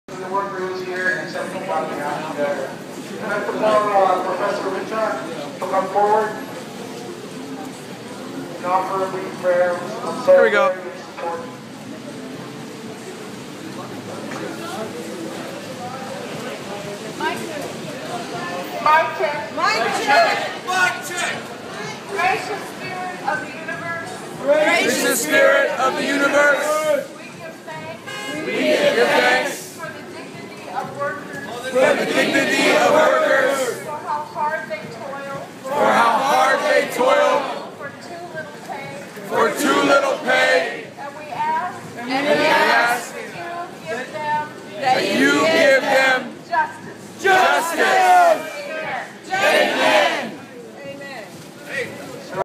Occupy SF: Interfaith Alliance Candlelight Vigil (raw)
raw footage of Interfaith Alliance marching from SF's Union Square to Occupy SF in Solidarity with the movement.